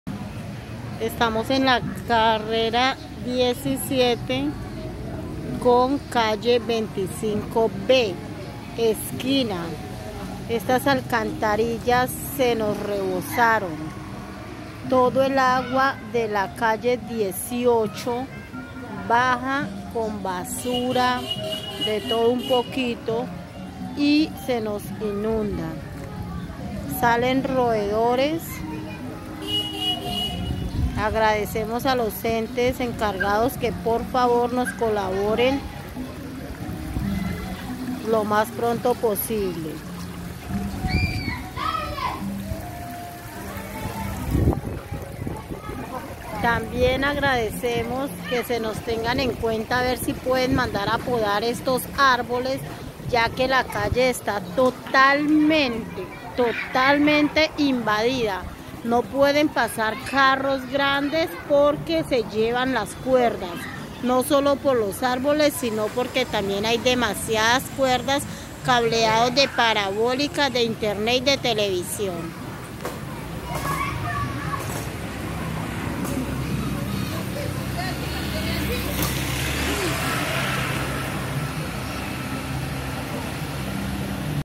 Inundada Cr 17 con Cll 25 B debido a que los sumideros de aguas lluvias en sector están taponados 1
Inundada la carrera 17 con calles 25 B en Yopal, debido a que los sumideros de aguas lluvias en el sector están taponados. Vecinos solicitan a la Secretaría de Obras y a la EAAAY solucionar el problema